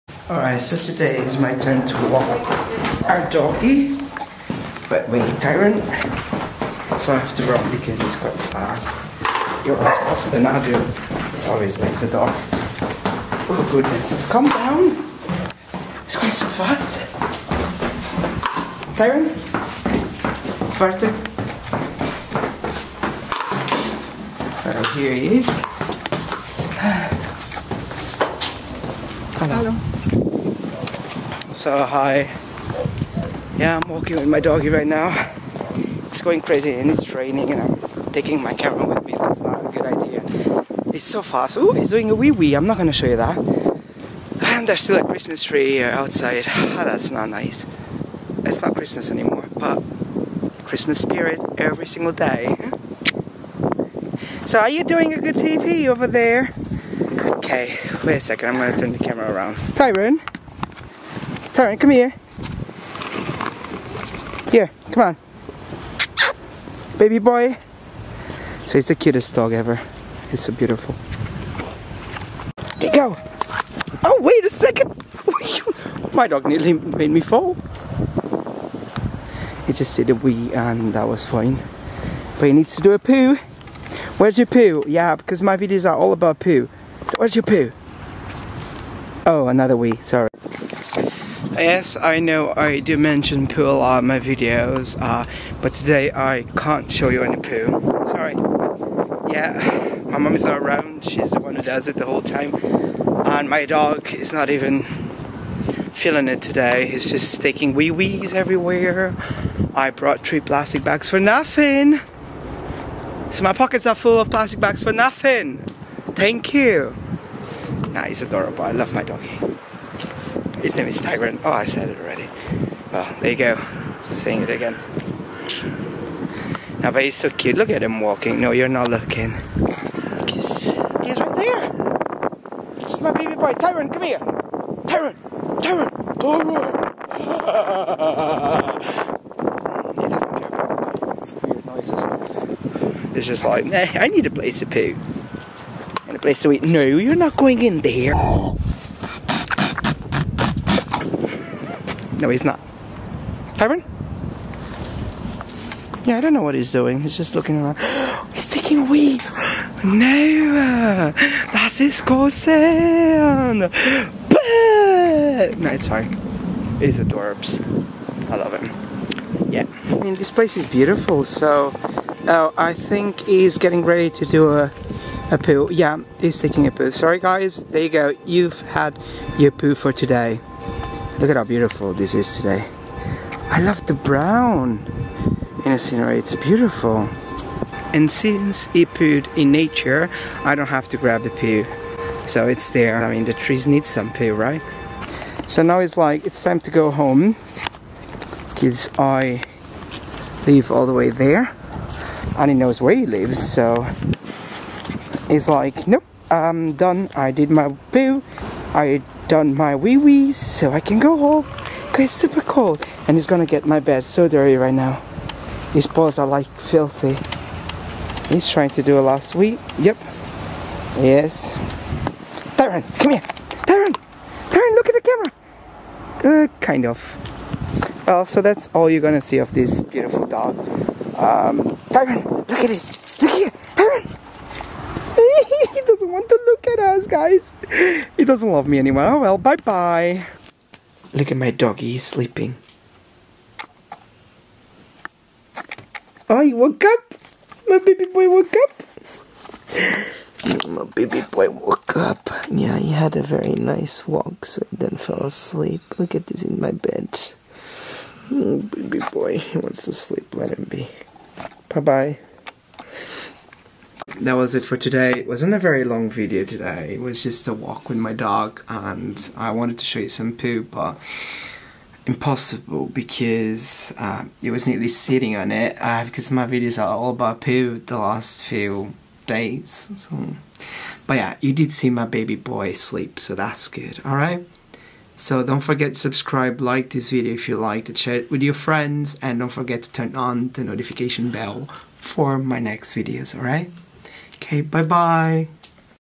A walk with my dog
I decided to vlog every single day for the whole year!!! CRAZY!
So, today I decided to be the one walking my baby boy Tyrone, and it's always fun when I do it myself, he's such a cutie, and... he ignores my existence completely.